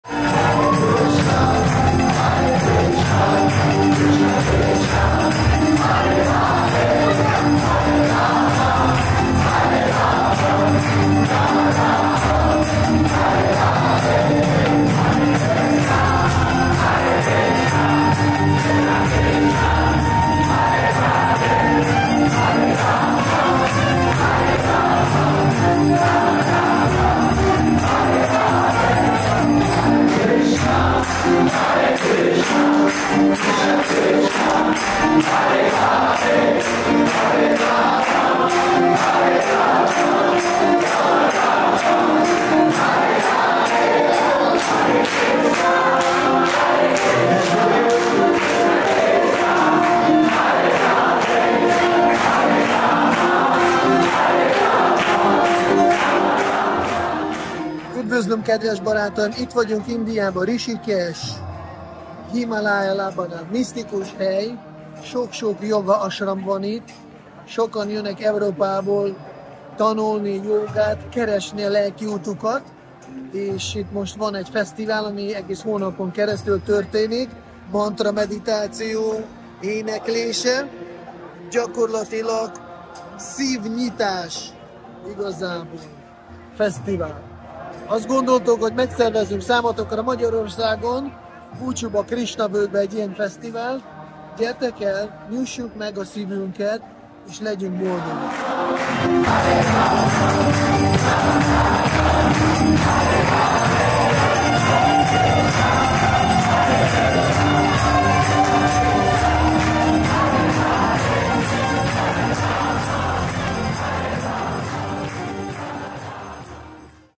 KIRTAN JAM Krisna-Völgy. Bhaktipāda Gosvāmī
Regisztrálj:

kjirtanjam.krisnsvolgy.hu

Spirituális megtapasztalások közös éneklés és táncolás közben: ez a Kírtan Jam

Kapcsolat a spirituális dimenzióval a zenén keresztül.
A spirituális energiákhoz legkönnyebben a zenén át lehet kapcsolódni.
Mindannyian vágyunk a lelki dimenzió megtapasztalására. Azonban a világunk mintha most szándékosan gátolna ebben bennüket. A Kirtan Jam előadói olyan művészek, akik képesek áttörni a materiális világ falait és átvinni másokat is a túloldalra.

Bhaktipāda Gosvāmī